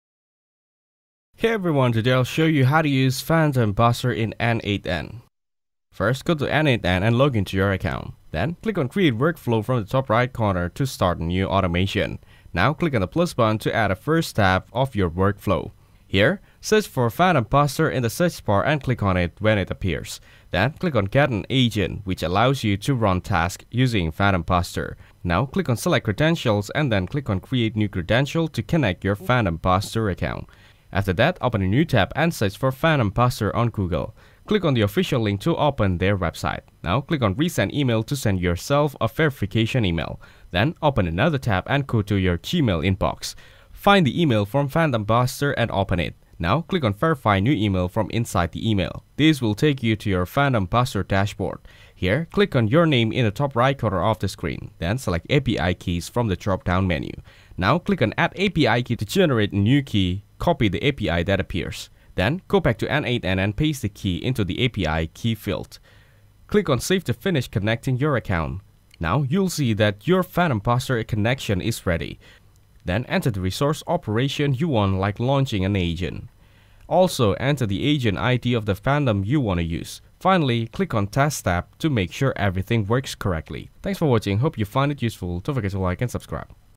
How to Connect PhantomBuster with N8N (Step-by-Step)
How to Connect PhantomBuster with N8N (Step-by-Step) Looking to connect PhantomBuster with N8N? In this step-by-step tutorial, we’ll walk you through exactly how to set up PhantomBuster integration in N8N for powerful automation workflows. Whether you're a marketer, growth hacker, or business owner, learning to connect PhantomBuster with N8N can help automate lead generation, web scraping, LinkedIn outreach, and more. By the end of this video, you'll be able to use PhantomBuster with N8N like a pro — fetching data from the web and sending it straight into Google Sheets, CRMs, or Slack.

This guide is beginner-friendly, and we’ll cover how to connect PhantomBuster to N8N using API keys, webhook setup, and how to trigger PhantomBuster automation flows right from N8N.